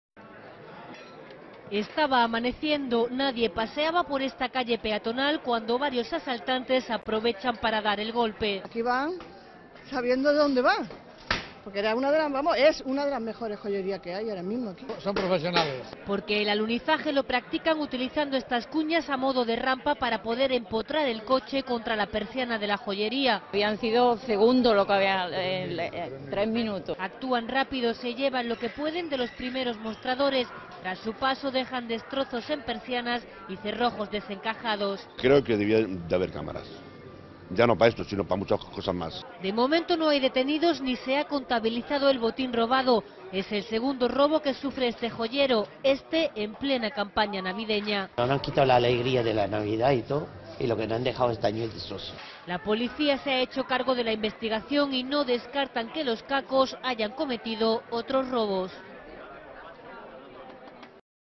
Robo en una joyería de Torremolinos
Estaba amaneciendo, nadie paseaba por una calle peatonal de Torremolinos cuando varios asaltantes aprovechan para dar el golpe. Porque el alunizaje lo practican utilizando unas cuñas a modo de rampa para poder empotrar el coche contra la persiana de la joyería. Actúan rápido y se llevan lo que pueden de los primeros mostradores. Tras su paso dejan destrozos en persianas y cerrojos desencajados. De momento no hay detenidos ni se ha contabilizado el botín robado. Es el segundo robo que sufre este joyero, éste en plena campaña navideña. La Policía se ha hecho cargo de la investigación y no descartan que los cacos hayan cometido otros robos.